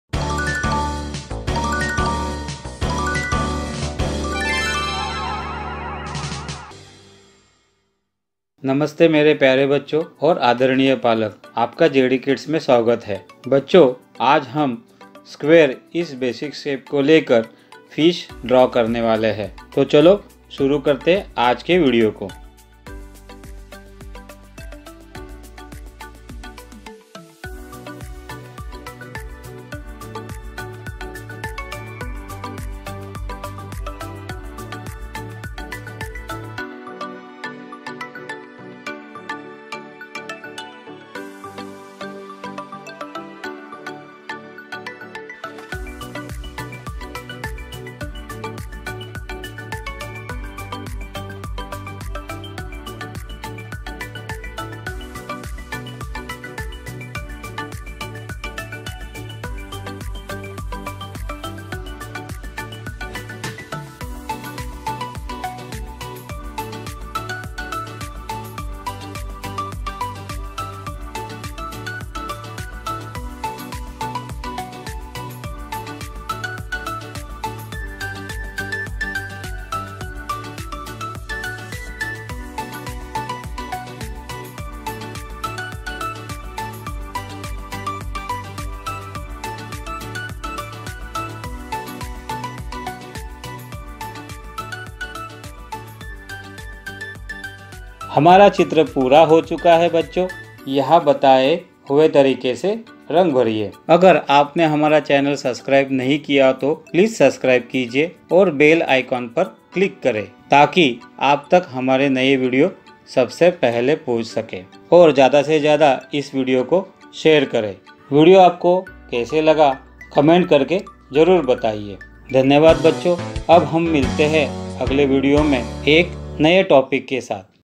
How To Draw Fish :::: Online Drawing Tutorials
Hello Friends!!!
Today I am showing you How To Draw Fish using different geometrical shapes.